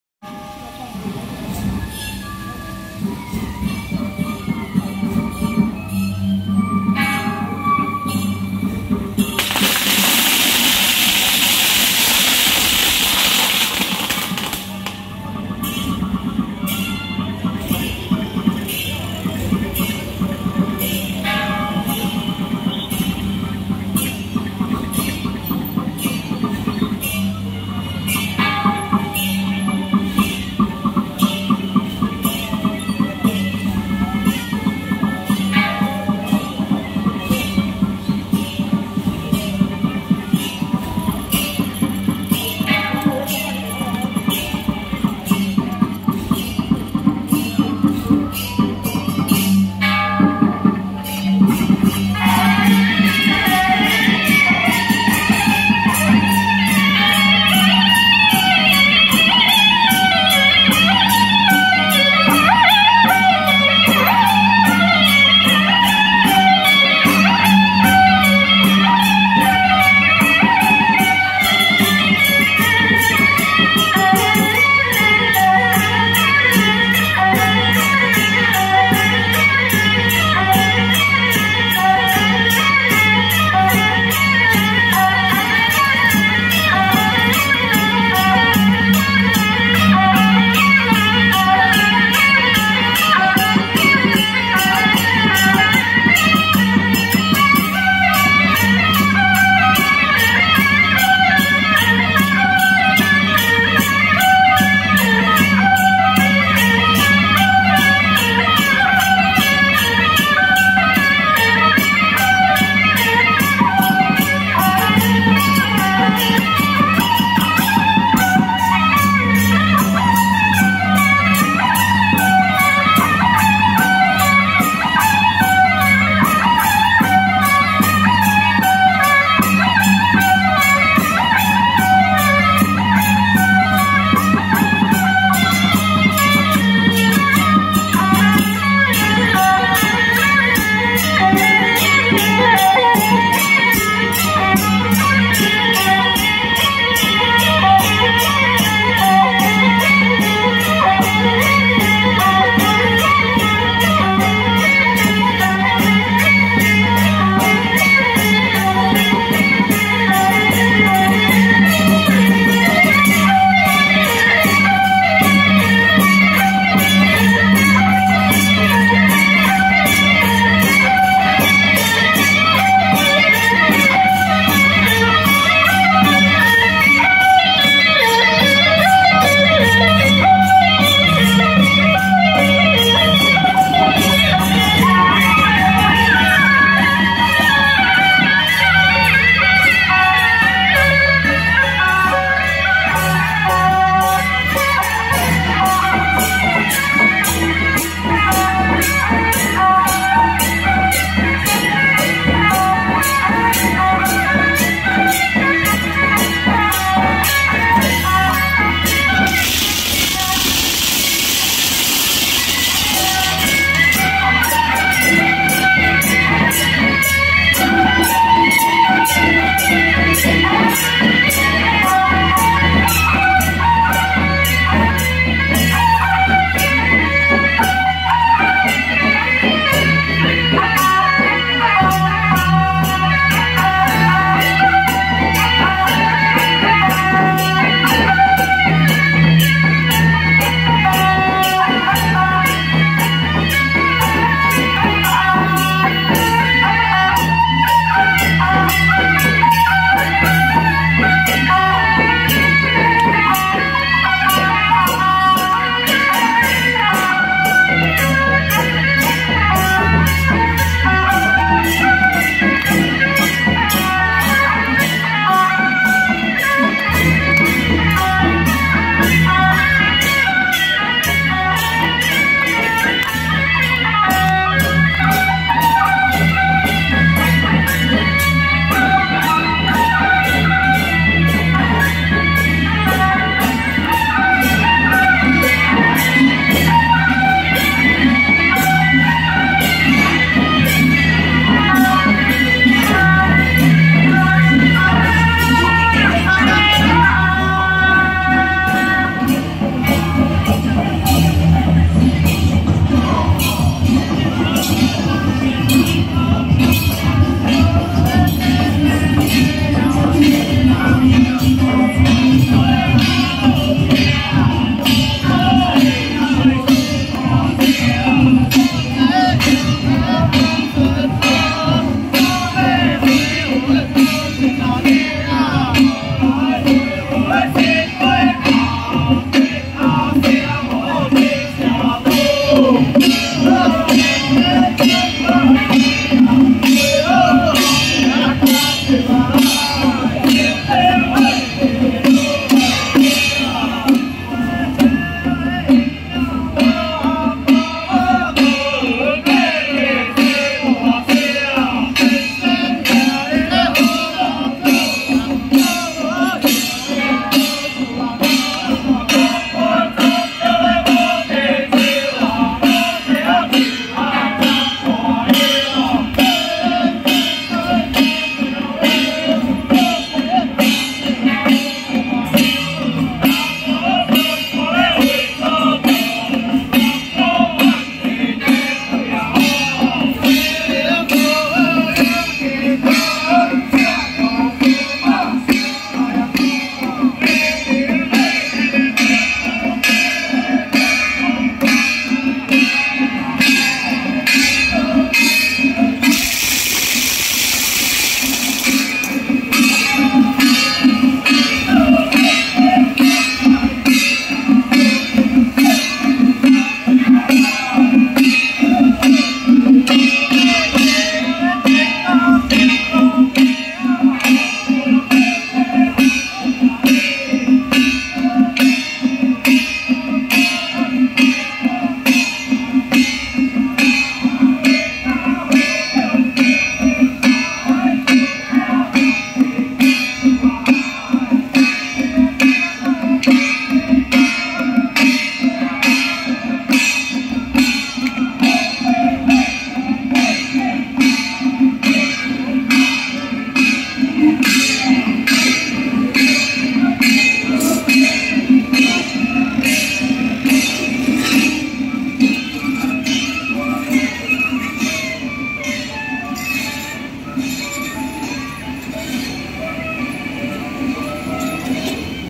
壬寅年高雄鳳邑三乃宮濟陽會吳府千歲進香回駕遶境 全隊伍 過境 鳳邑鎮安宮 [廟會紀實]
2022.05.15 天運歲次壬寅年 農曆四月十五日 高雄鳳邑三乃宮濟陽會吳府千歲 前往 嘉義東石港臨水宮 東石港先天宮 進香回駕遶境大典

拍攝: 廟會紀實